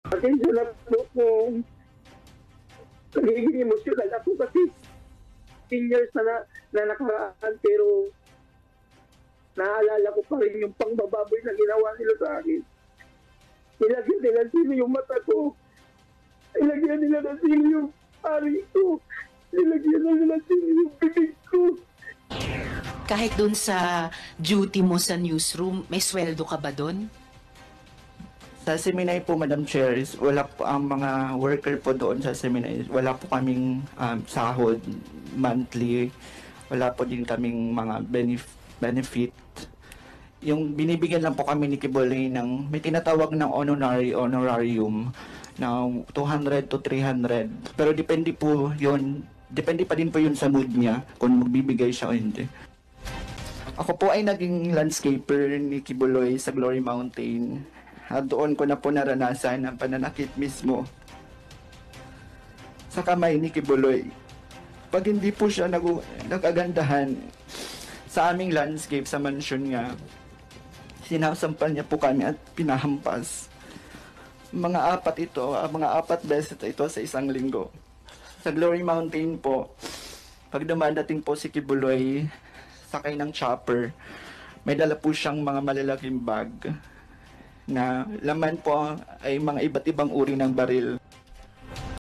Alyas David & Alyas Rene Imbestigasyon ng Senado sa mga alegasyon ng pang-aabuso ni Pastor Quiboloy
Alyas David & Alyas Rene - Imbestigasyon ng Senado kaugnay sa mga alegasyon ng pang-aabuso sa kamay ni Kingdom of Jesus Christ Leader Pastor Apollo Quiboloy, ipinagpatuloy kahapon